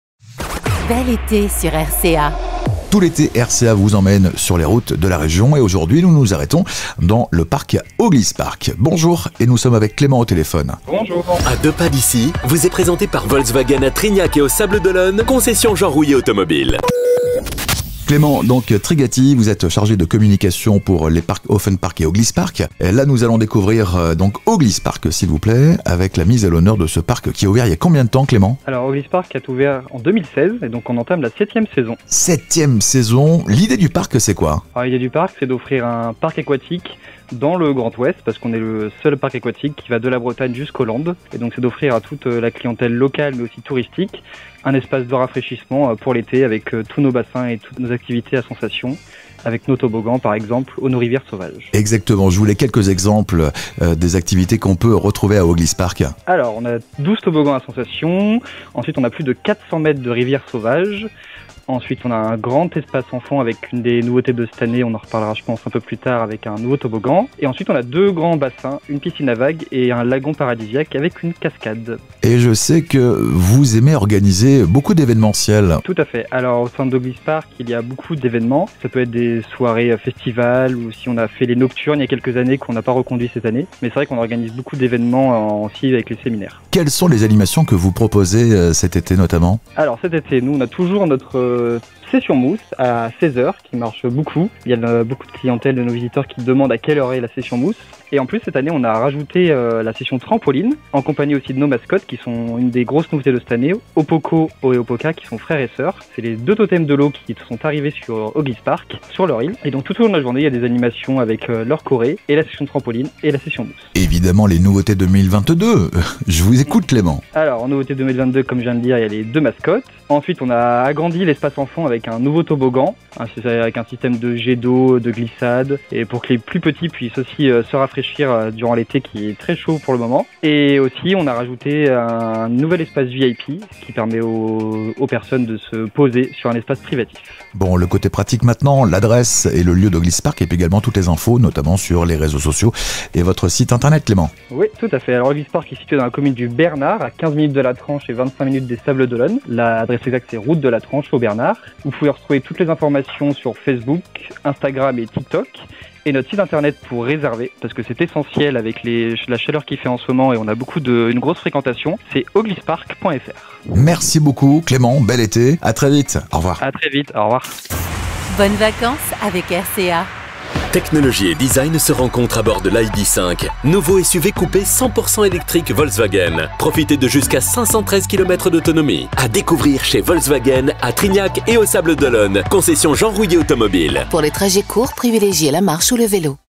À Deux Pas D'Ici - O'Gliss Park
Direction la Vendée à la découverte de O'Gliss Park !
A seulement quelques minutes des plages vendéennes, O'GLISS PARK est un parc aquatique situé au cœur d'une oasis de détente. Idéal pour se « la couler douce » ou pour vivres des sensations fortes durant une journée entière, vous allez découvrir des univers et des ambiances qui sauront vous apporter tout ce que vous recherchez ...
Sensations, tranquillité, détente, moments en famille... Ou tout simplement un bon rafraichissement...